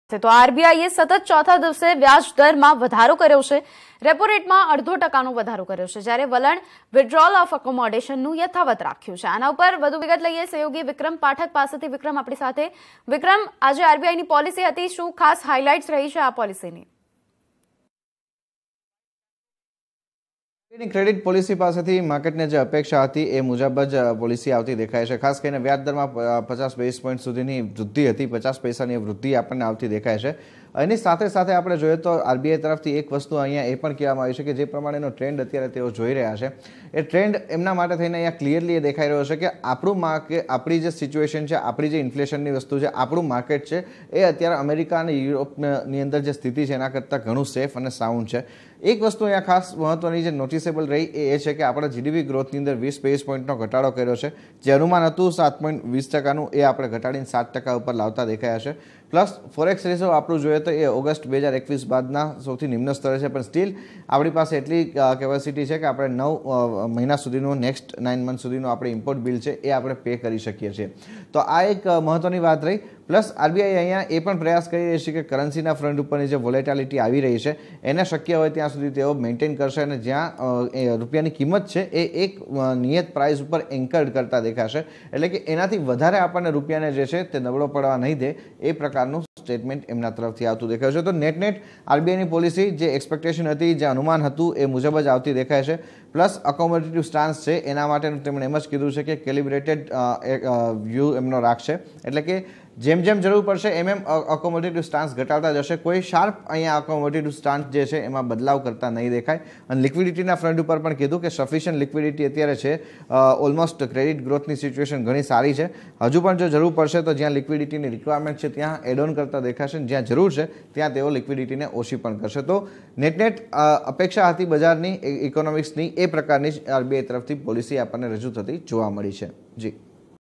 RBI hiked rates by 0.50% | Credit Policy | Rate Hike | Intrest Rate
CNBCBajar | RBI એ સતત ચોથી વખત વ્યાજ દરમાં વધારો કર્યો, રેટમાં અડધો ટકાનો વધારો કર્યો જ્યારે વલણ Withdrawal of Accomodation નું યથાવત્ રાખ્યું તો CreditPolicy ની તમામ જાણકારી આપી રહ્યાં છે વિક્રમ પાઠક